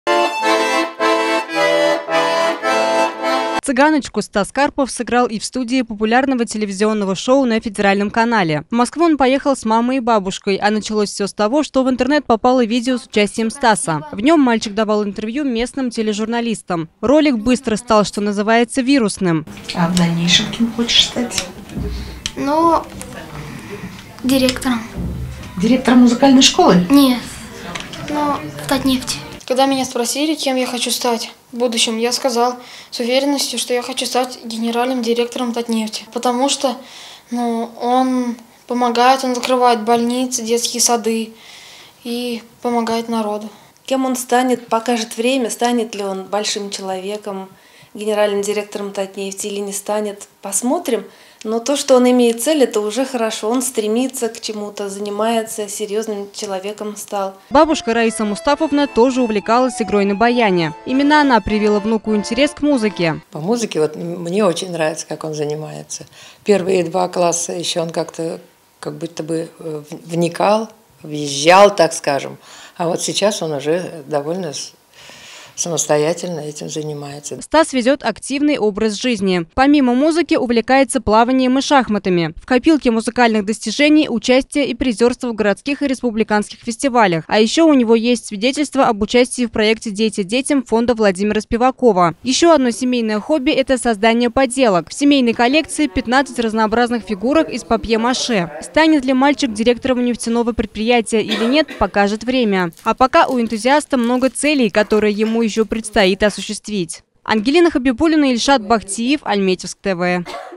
Пятиклассника из Альметьевска пригласили в Москву на съемки шоу
Альметьевский школьник попал на федеральный канал.